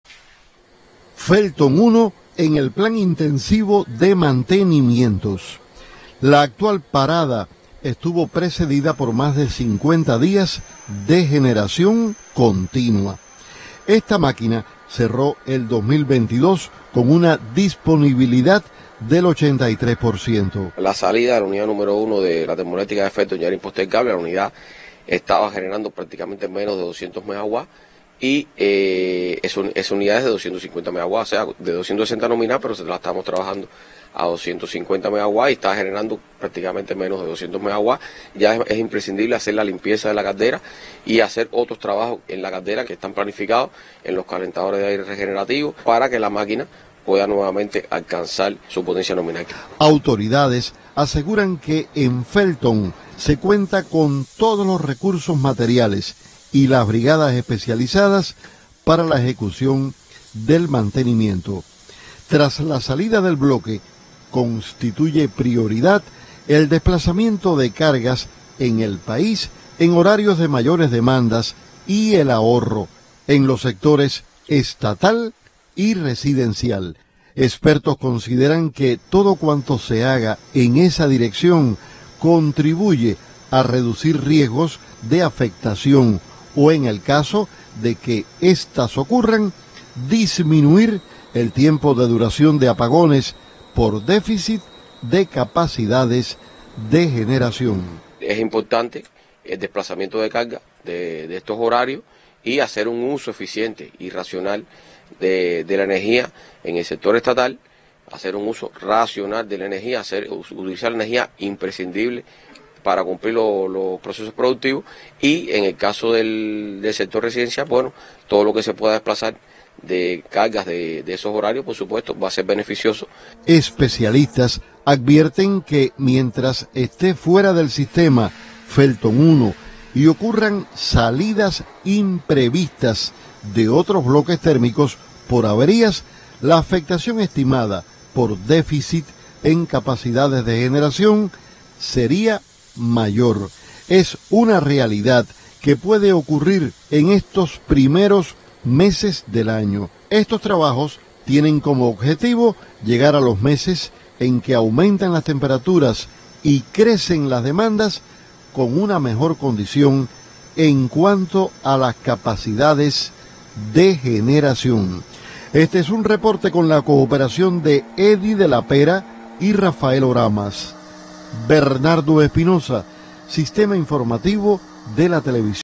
Cuba: Comienza mantenimiento al Bloque 1 de CTE de Felton
Comenzó el mantenimiento en el bloque UNO de la Termoeléctrica “Lidio Ramón Pérez” de Mayarí, en el oriente de Cuba. Pasado el mediodía de este martes, empezó el lavado del condensador, y en la noche se iniciará la limpieza de la caldera. La máquina recibe una intervención planificada de 20 días, para solucionar limitaciones tecnológicas y restablecer su capacidad nominal de generación.